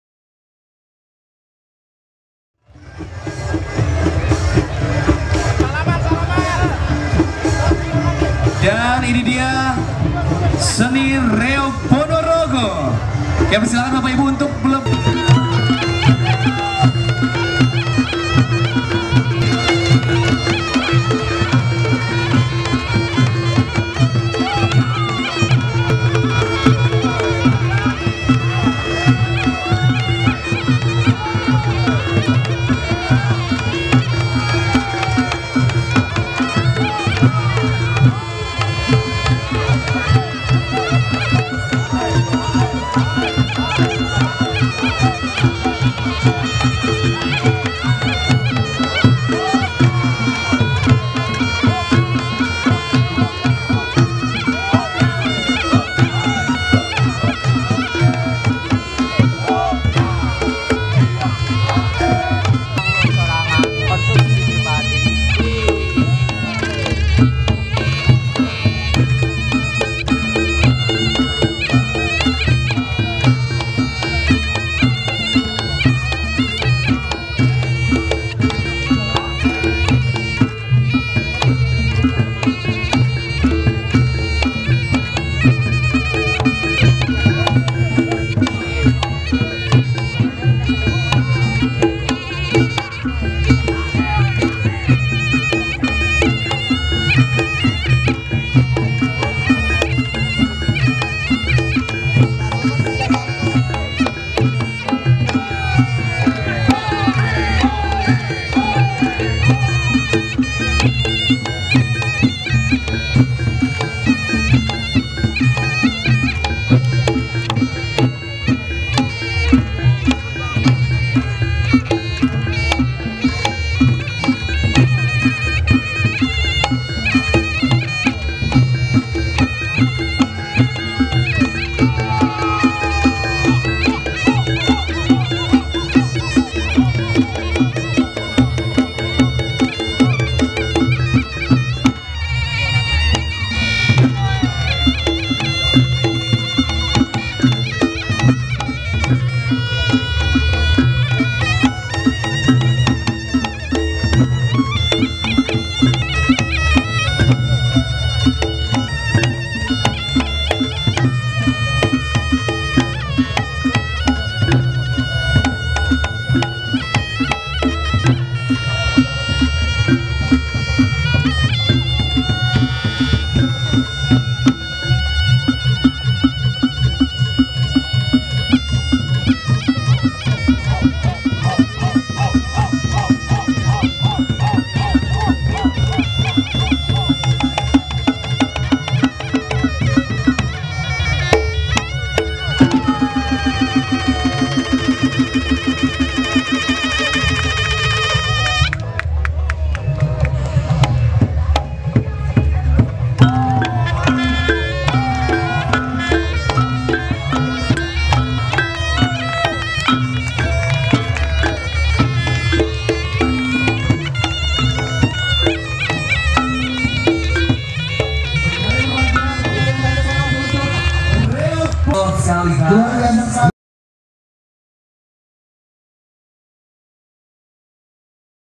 REOG PONOROGO SOHOR DI DUNIA HADIR DI PURWAKARTA || HUT PURWAKARTA 2019
Rangkaian Acara Milangkala Kota Purwakarta Ka 188 & Kabupaten Purwakarta Ka 51 Festival Ngarak Awug Rekor Muri Dunia.